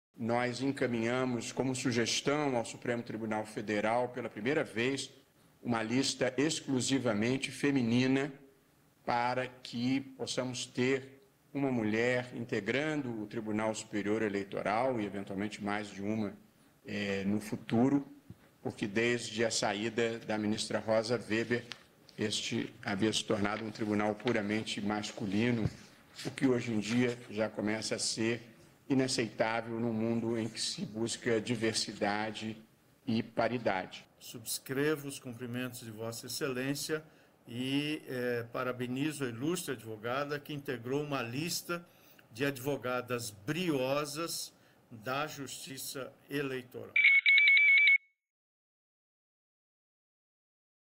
Ministros parabenizam nomeação de ministra substituta do TSE
Na sessão desta quinta-feira, o presidente do Tribunal Superior Eleitoral, ministro Luís Roberto Barroso, e o ministro Edson Fachin manifestaram boas-vindas à advogada Maria Claudia Bucchianeri, nomeada ministra substituta do TSE. A vaga a ser ocupada pela jurista foi aberta com a nomeação de Carlos Horbach como ministro efetivo da Corte, em maio deste ano.